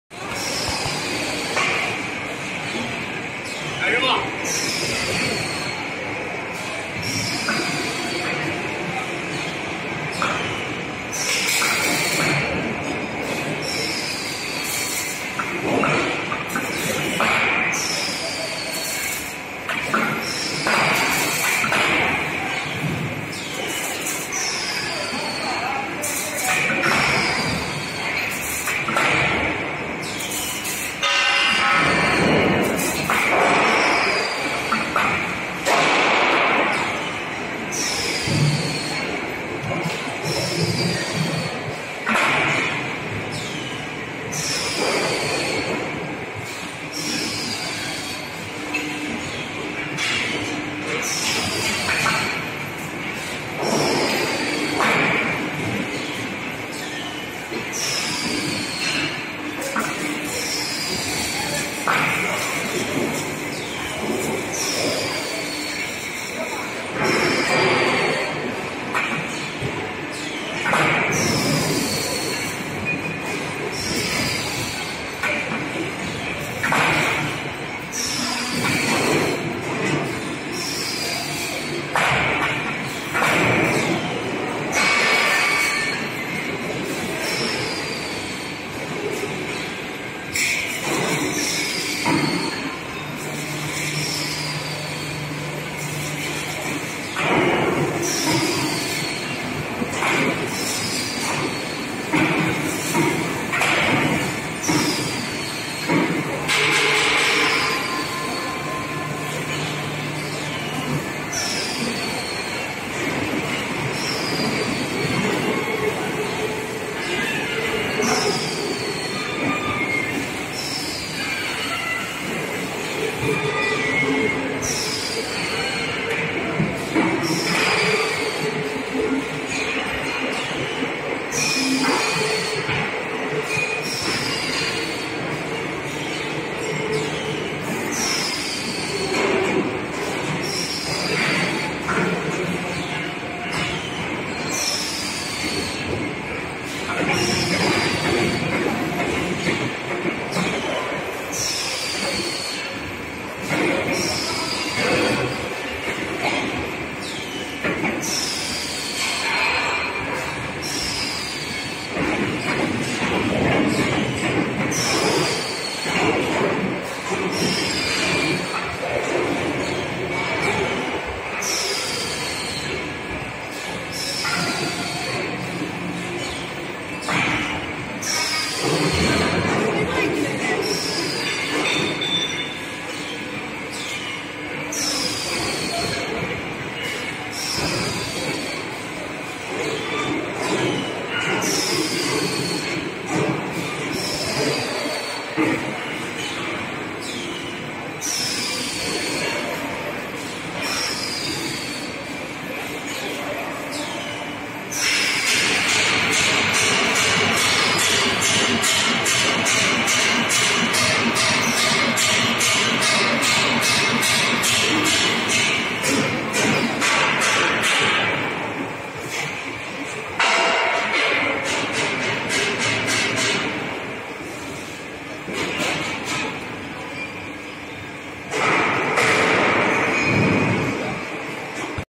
Double pots mixer
Double pots mixer is one of necessary equipment for making cutting discs, which is used to mix the raw materials evenly.